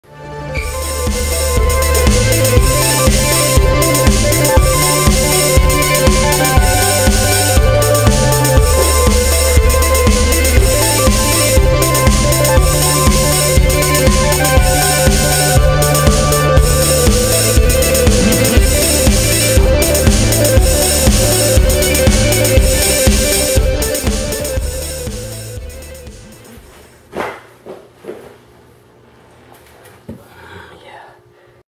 New Roll? - Storm Roll!
Yesterday I landed a new roll (for me) and was wondering if anyone's seen this one before?  It's now called the Storm Roll (thanks Eli)!